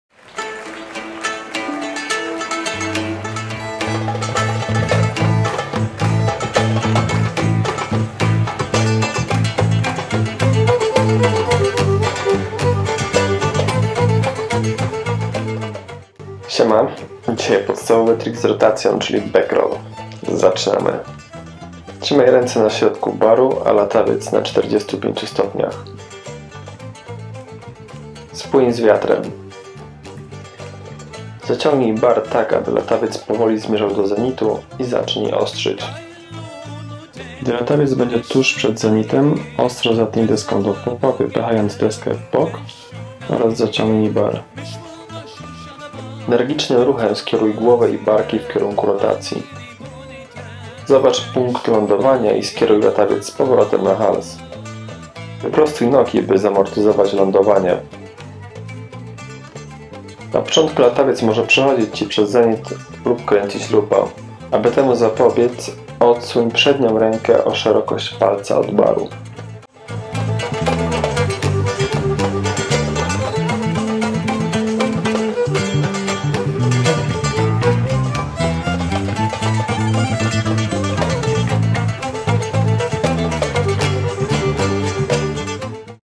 Kitesurfing: Jak zrobić backroll?
Tym razem uczymy się pierwszego tricku z rotacją, czyli backrolla.